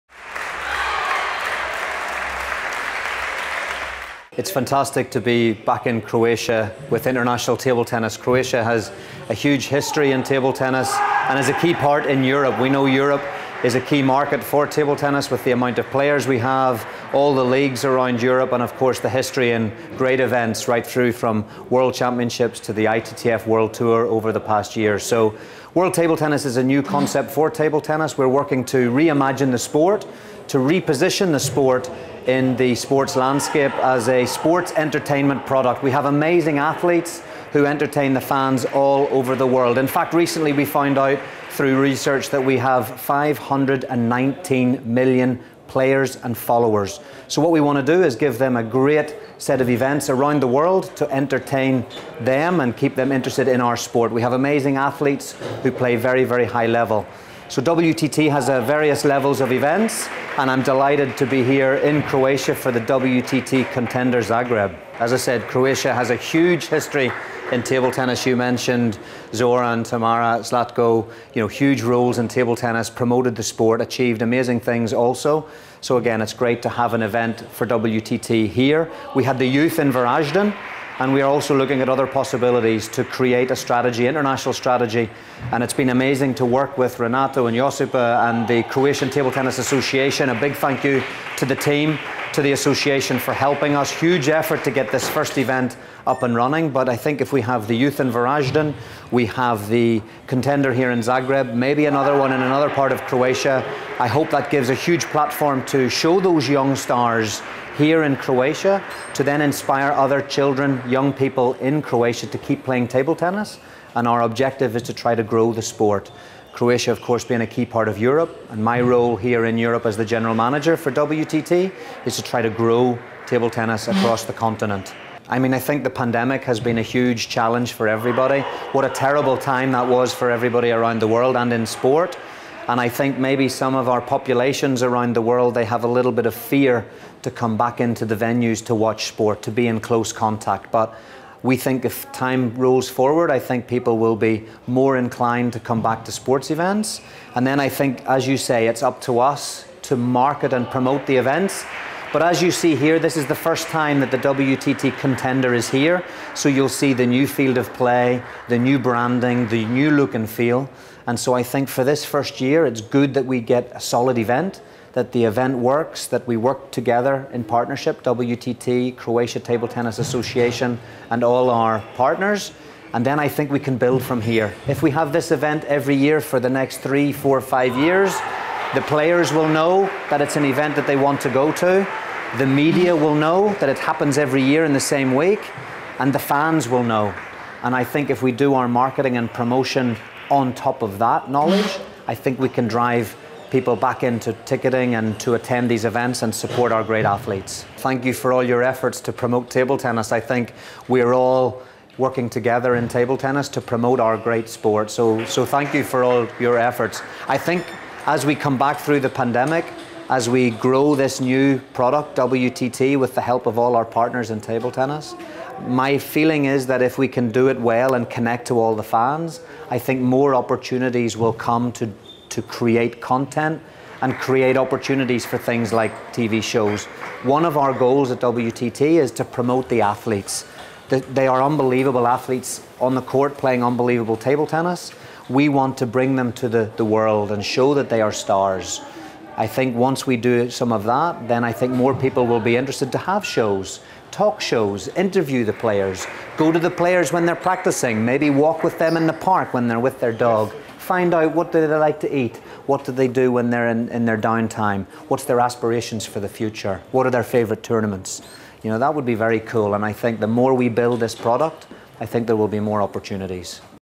WTT Contender Zagreb 2022 - Jonny Cowan
Zagreb as a Capital of Croatia is proud to be a part of WTT Contender family.  As it is known, Croatian table tennis has a long and rich history. And since many passionate table tennis fans around the world are deprived of the biggest live events, we are really happy to be able to watch the best players in our city in our city. Therefore, thanks to WTT for the opportunity, as well as special thanks to Mr. Jonny Cowan for this interview.